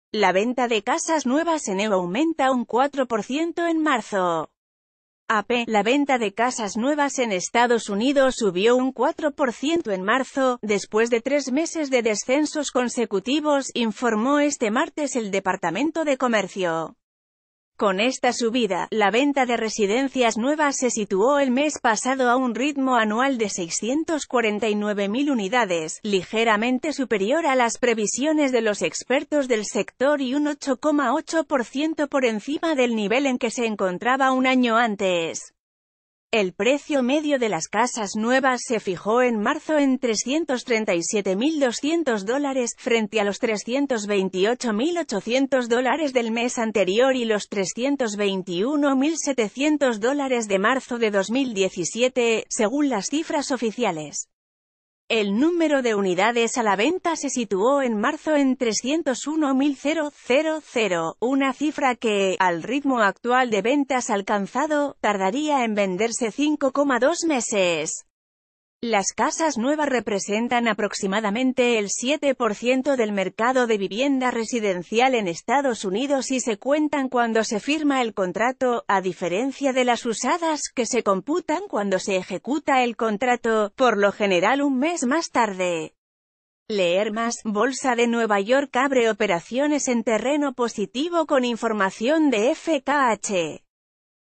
La venta de casas nuevas en EU aumenta un 4% en marzo
La venta de casas nuevas en EU aumenta un 4% en marzo. (AP) La venta de casas nuevas en Estados Unidos subió un 4% en marzo, después de tres meses de descensos consecutivos, informó este martes el Departamento de Comercio. Con esta subida, la venta de residencias nuevas se situó el mes pasado a un r...